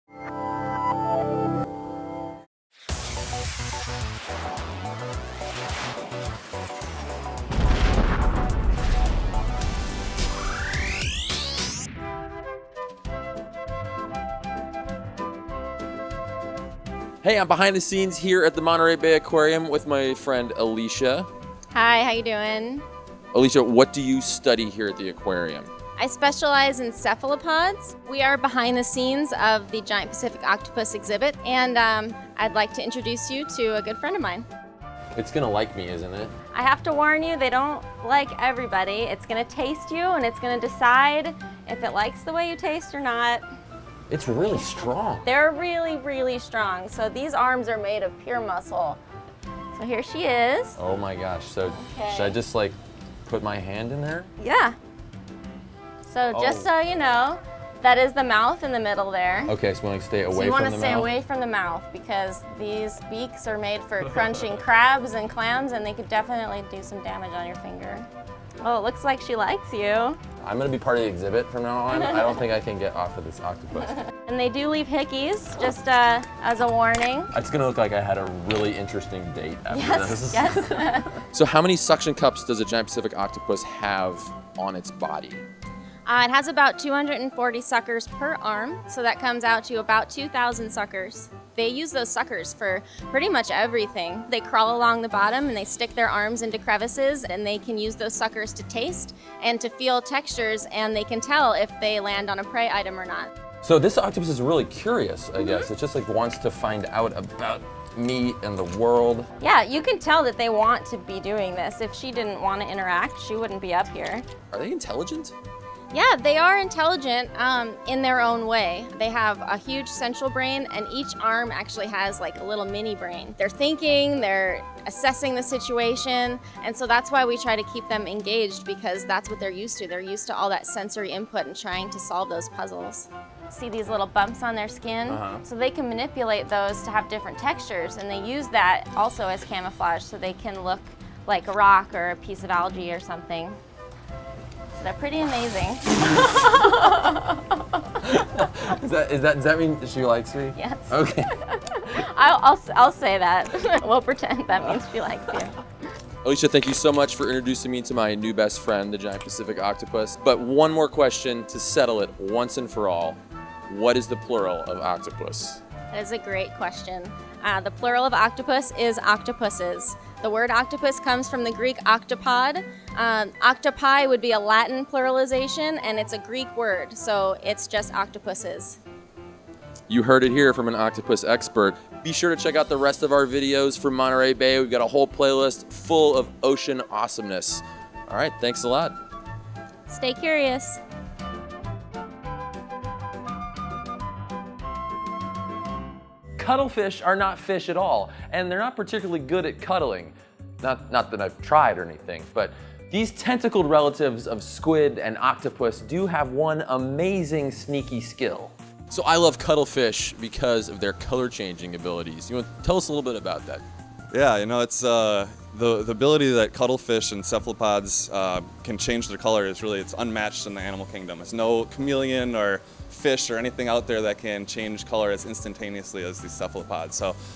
My Date With A Giant Pacific Octopus!
More below:

Joe went behind the scenes at Monterey Bay Aquarium to meet the Giant Pacific Octopus, a powerful and curious creature that challenges many of our notions of intelligence!